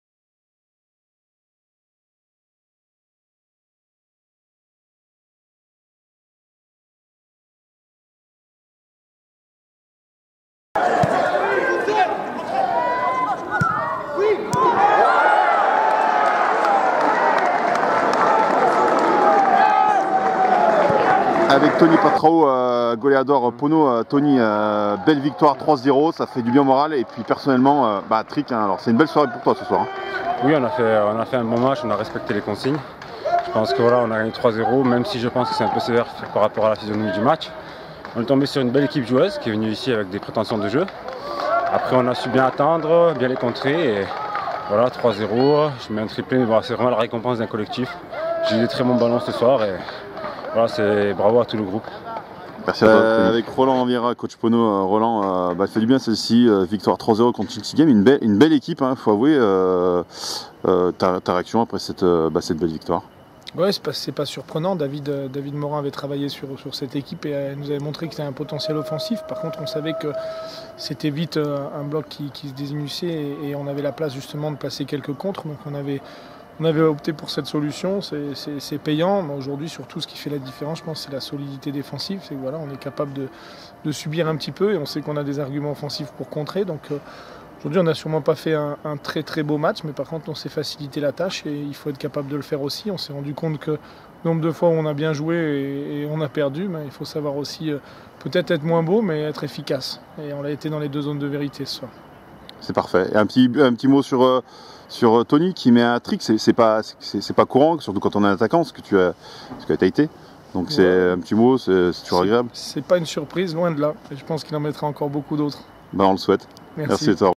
18/11/2017 N2: Le Puy Foot 3 - 0 Schiltigheim
Le 3ème but et les réactions !